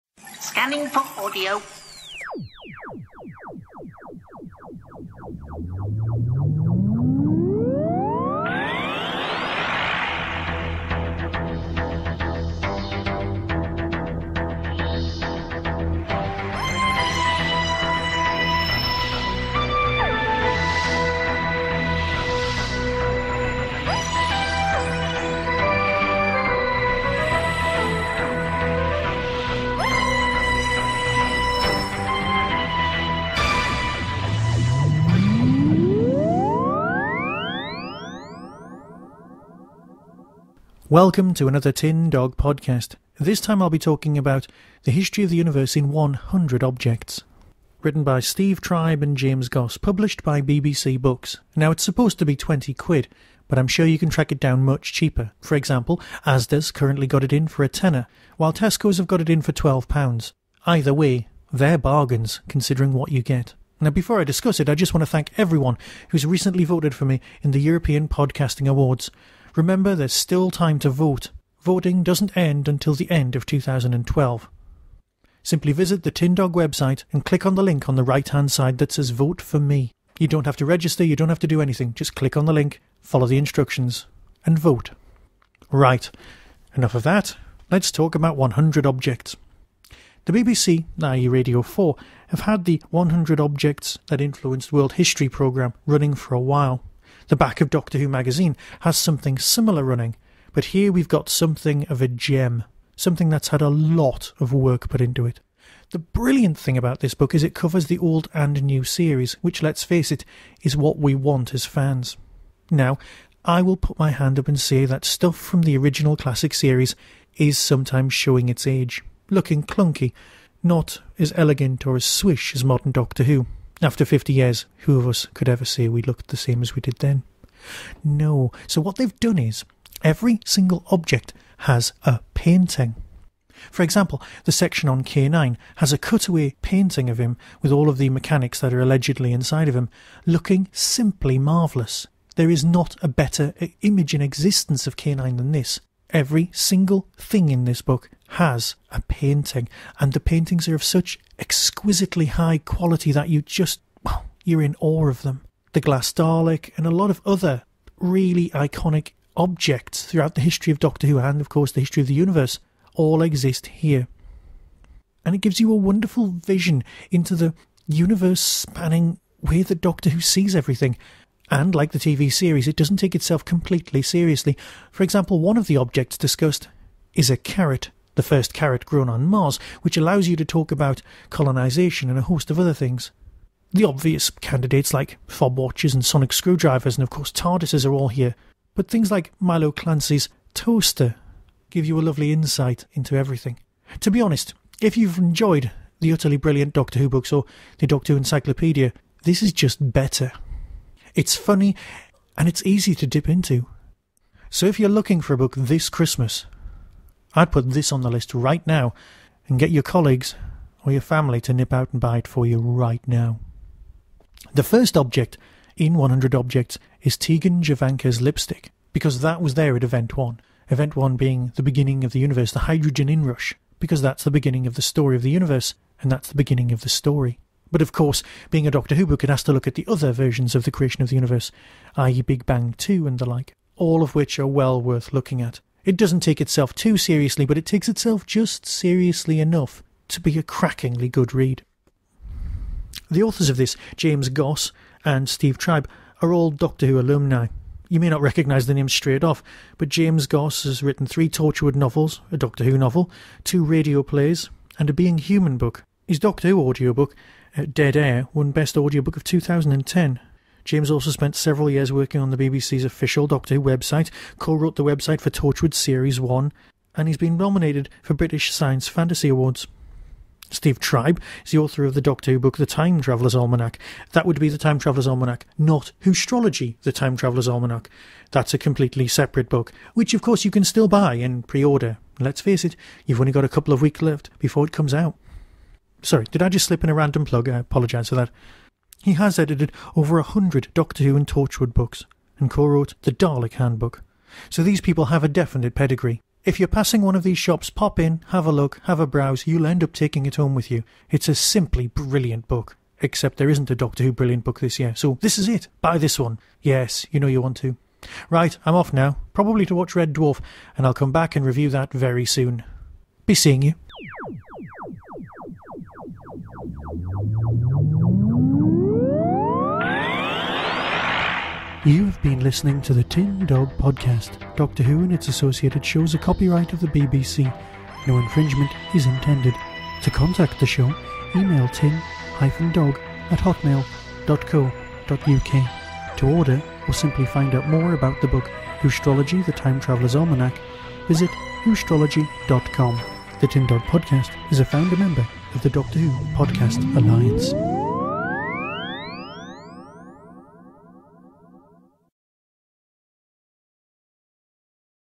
TDP 275: The History of the Universe in 100 Objects
Every object tells a story. From ancient urns and medieval flasks to sonic screwdrivers and glass Daleks, these 100 objects tell the story of the entire universe, and the most important man in it: the Doctor.

Each item has a unique tale of its own, whether it's a fob watch at the onset of the Great War or a carrot growing on the first human colony on Mars. Taken together, they tell of empires rising and falling, wars won and lost, and planets destroyed and reborn.

Within these pages lie hidden histories of Time Lords and Daleks, the legend of the Loch Ness Monster, the plot to steal the Mona Lisa and the story of Shakespeare's lost play. You'll find illustrated guides to invisible creatures, the secret origins of the internet, and how to speak Mechonoid.

A History of the Universe in 100 Objects is an indispensible guide to the most important items that have ever existed, or that are yet to exist.